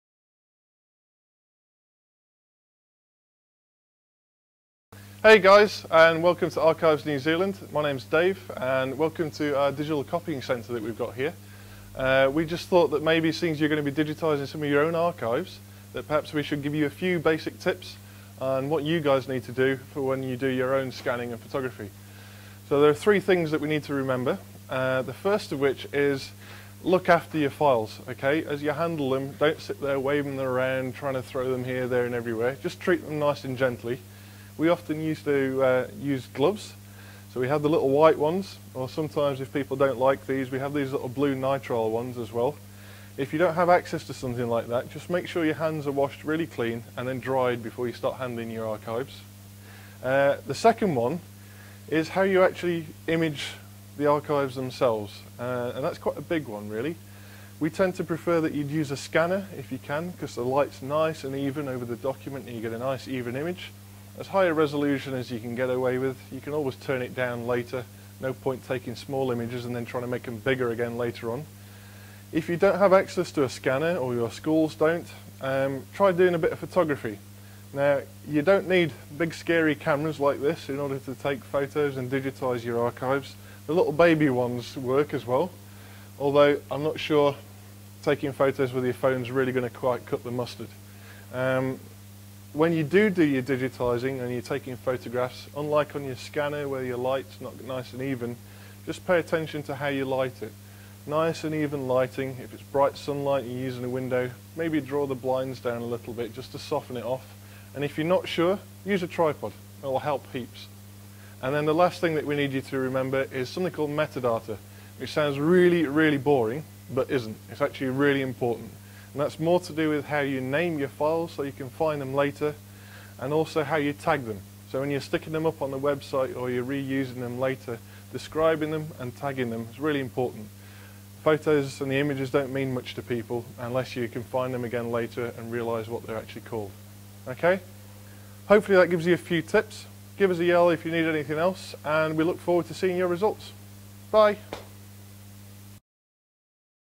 Digitisation Tips and Tricks
Short video giving a few tips and tricks on basic digitisation of your own archives.